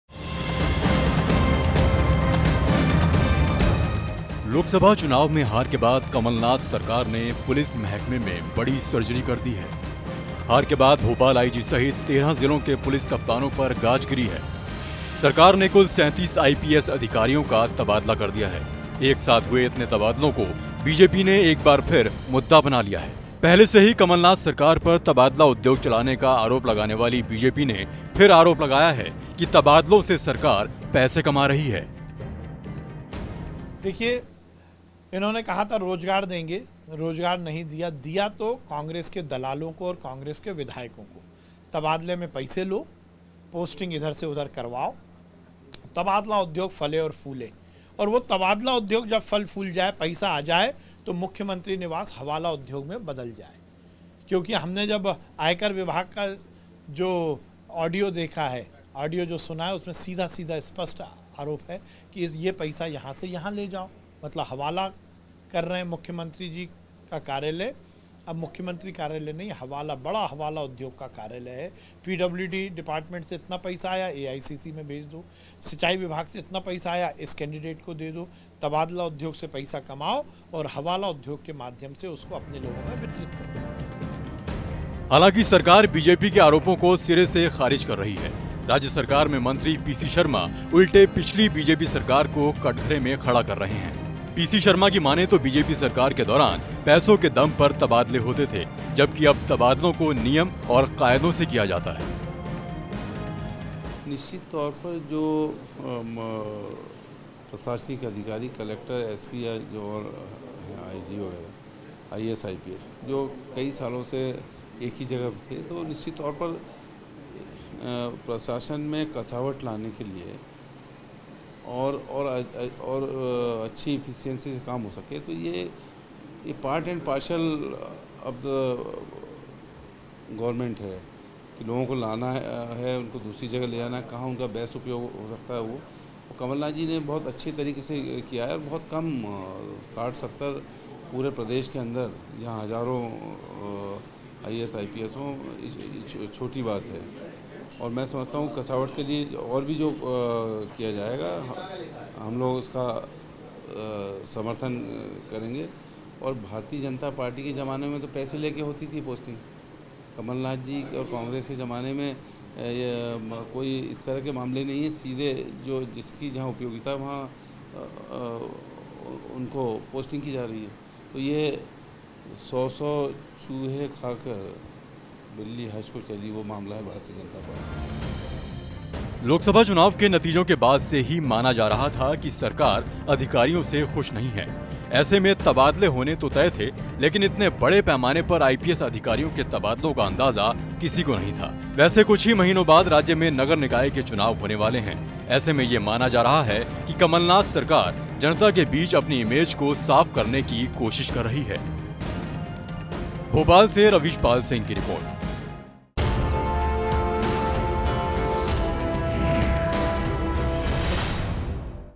तबादलों को लेकर फिर घिर गई Kamalnath सरकार !|MPTak
लोकसभा चुनाव में हार के बाद कमलनाथ सरकार ने पुलिस महकमे में बड़ी सर्जरी कर दी है. हार के बाद भोपाल आईजी सहित 13 जिलों के पुलिस कप्तानों पर गाज गिरी है. सरकार ने कुल 37 आईपीएस अधिकारियों का तबादला कर दिया है. एक साथ हुए इतने तबादलों को बीजेपी ने एक बार फिर मुद्दा बना लिया है...पहले से ही कमलनाथ सरकार पर तबादला उद्योग चलाने का आरोप लगान वाली बीजेपी ने फिर आरोप लगाया है कि तबादलों से सरकार पैसे कमा रही है।